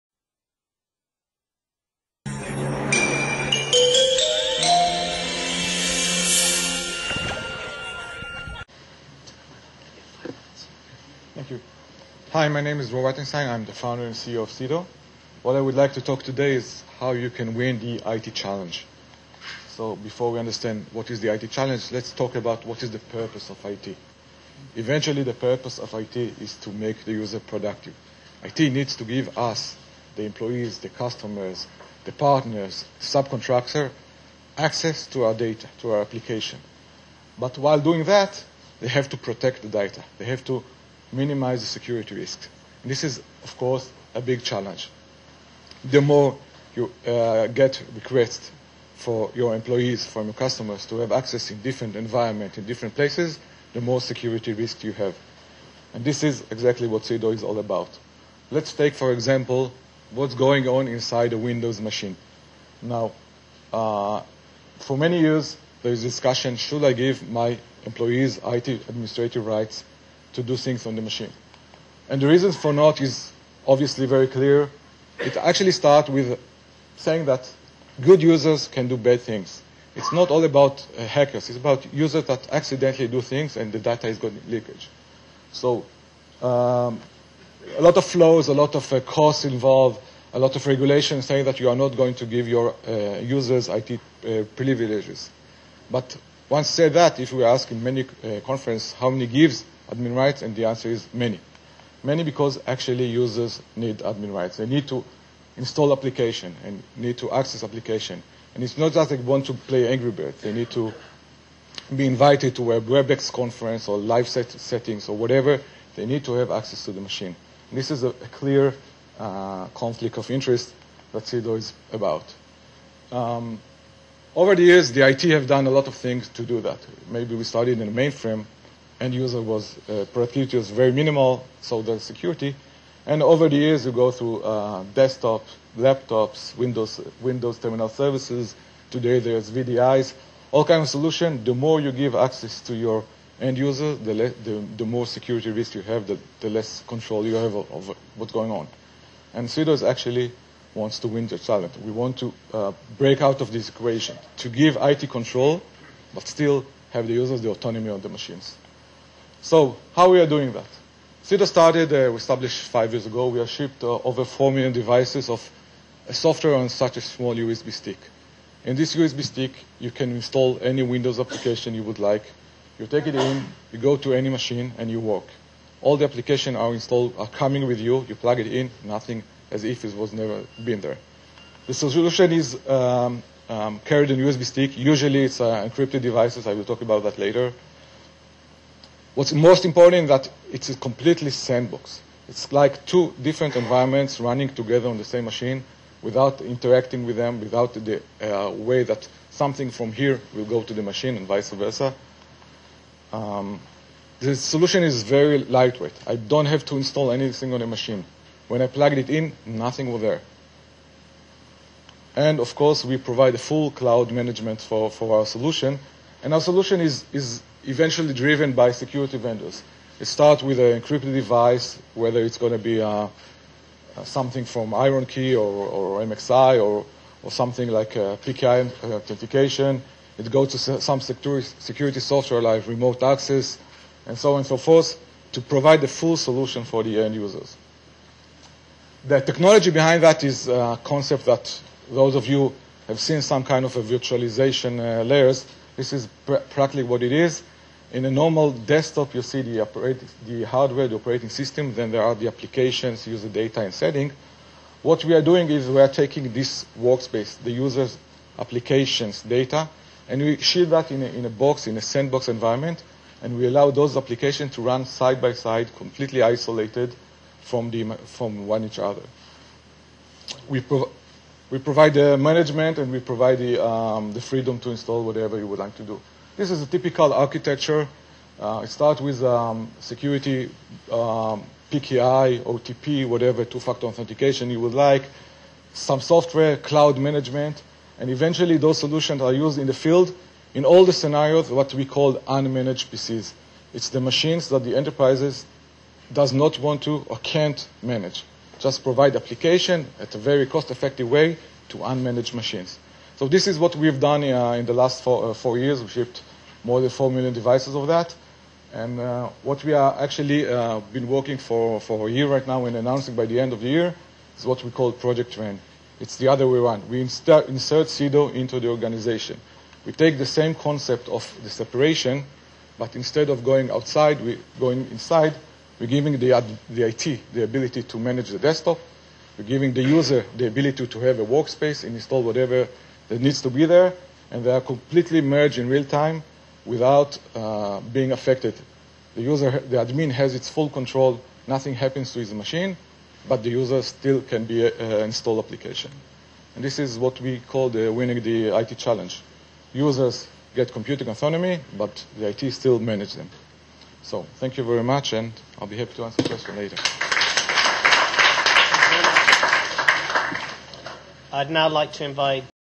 Presentation by Ceedo "Winning the IT Challenge", Israel
Lecturer: Dror Wettenstein, Founder and CEO
Global Security Challenge 2011
"Penetrating, Partnering, Achieving: Go to Market Strategies and Innovation in HLS"
Global Security Challenge Europe and Middle East Semifinals (500,000$)
U.S. Department of Defense Initiative
Held at the MIT Enterprise Forum,
Tel Aviv University, Israel
21/9/11